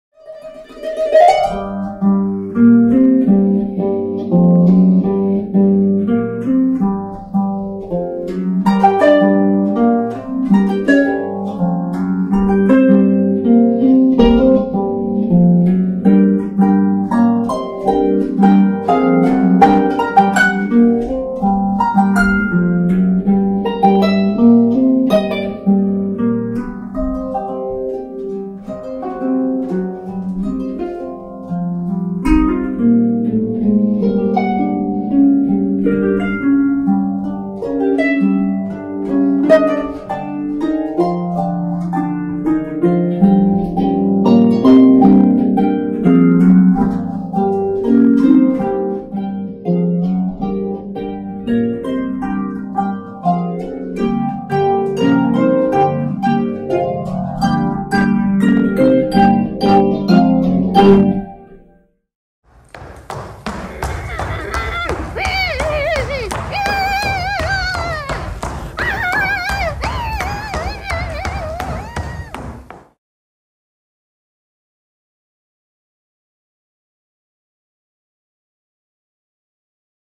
Mission Harpossible_English Teaser
Two talented harpists perfectly complicit but totally competing in a show unbridled, astonishing, burlesque and ... melodious!